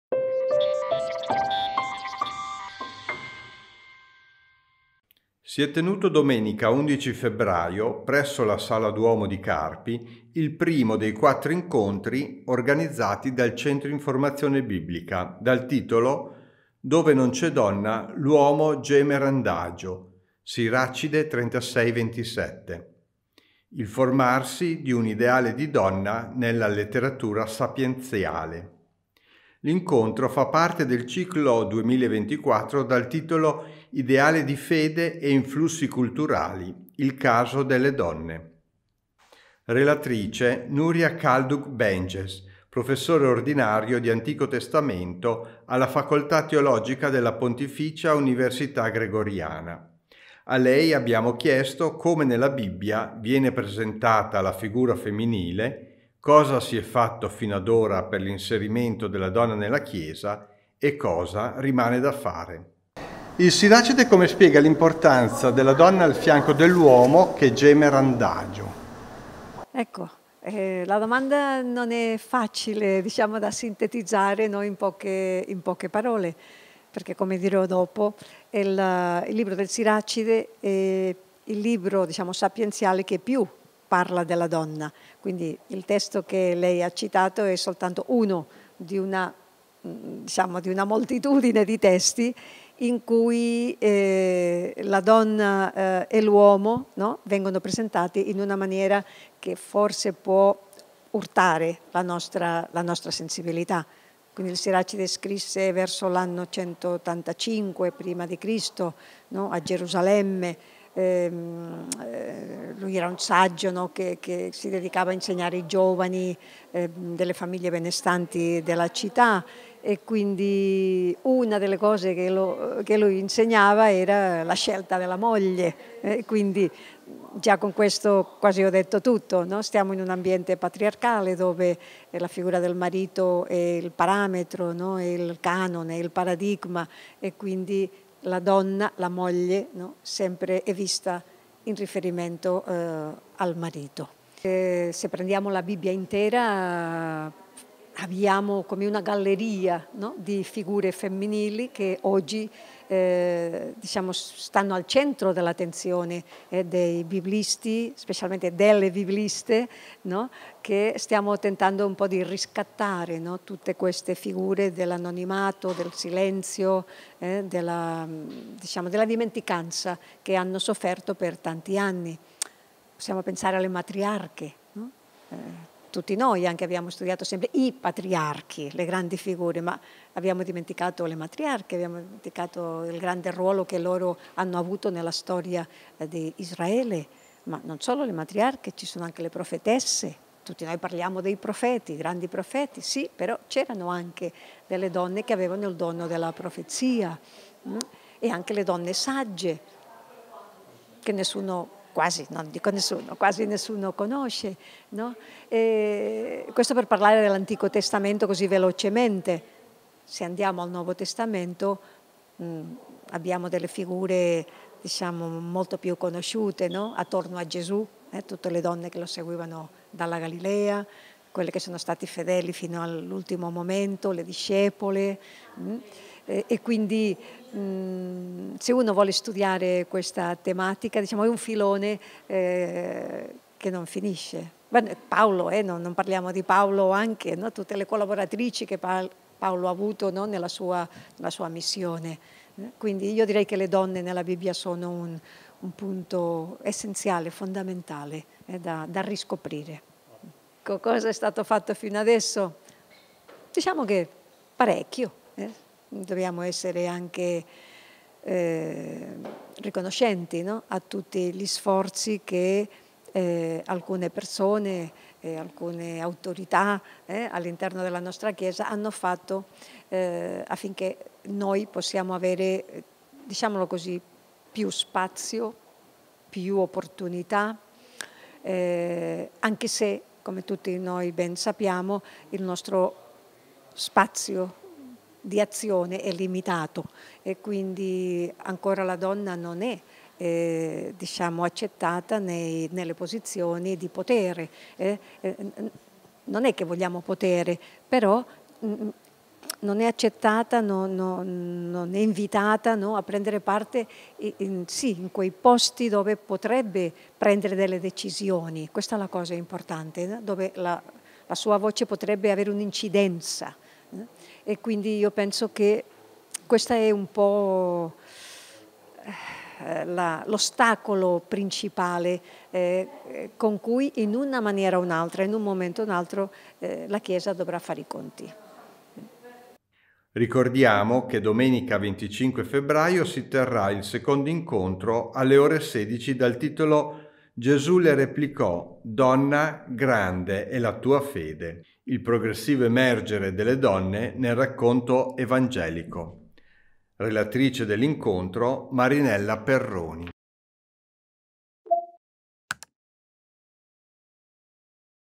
Intervista a Nuria Calduch Benages
Si è tenuto  domenica 11 febbraio 2024, presso la Sala Duomo di Carpi, il primo dei quattro incontri organizzati dal Centro Informazione Biblica dal titolo “Dove non c’è donna, l’uomo geme randagio” (Sir. 36,27), il formarsi di un ideale di donna nella letteratura sapienzale. L’incontro fa parte del Ciclo 2024 dal titolo “Ideale di Fede e influssi culturali. - Il caso delle Donne – “ Relatrice  Nuria Calduch Benages Professore ordinario di Antico Testamento della Facoltà Teologica della Pontificia Università Gregoriana. A lei abbiamo chiesto come nella bibbia viene presentata la figura femminile, cosa si è fatto fino ad ora per l’inserimento della donna nella Chiesa e cosa rimane da fare.